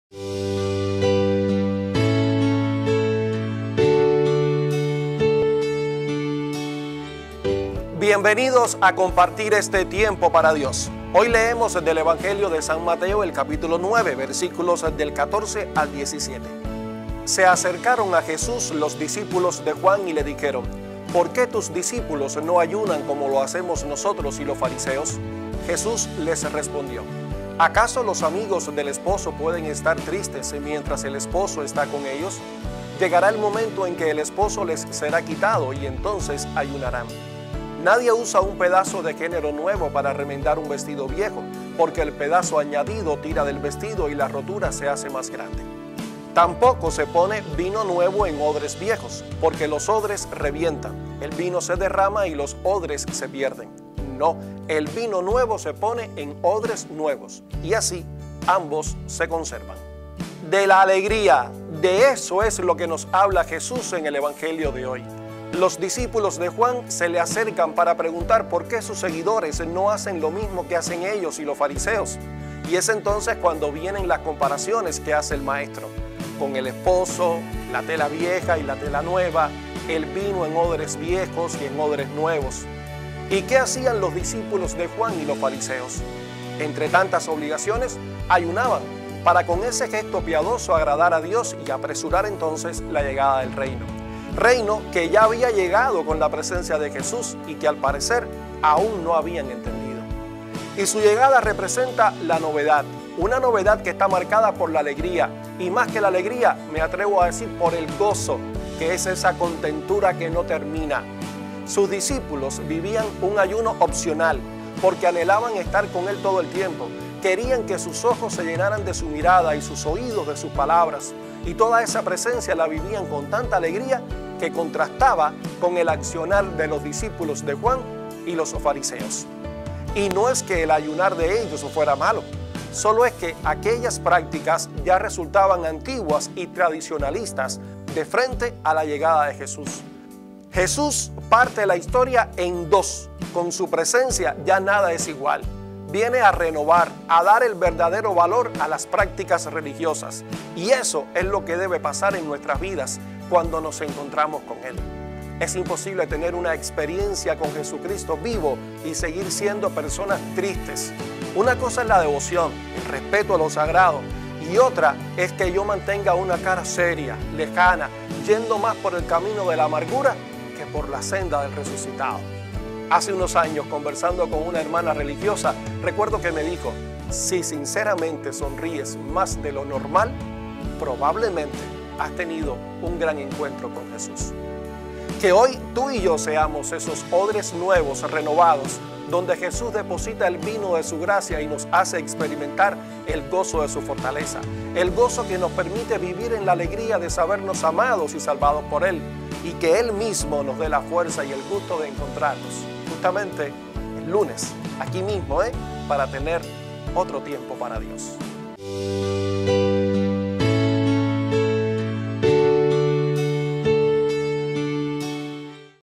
EVANGELIO DE HOY. SABADO 4 DE JULIO SAN MATEO 9, 14-17
Nelson de la Rosa católico y SiervodeCristoVivo, comparte con nosotros el EvangelioDeHoy

De la alegría, de eso es lo que nos habla Jesús en el Evangelio de hoy.
Hace unos años, conversando con una hermana religiosa, recuerdo que me dijo: “Si sinceramente sonríes más de los normal probablemente has tenido un gran encuentro con Jesús.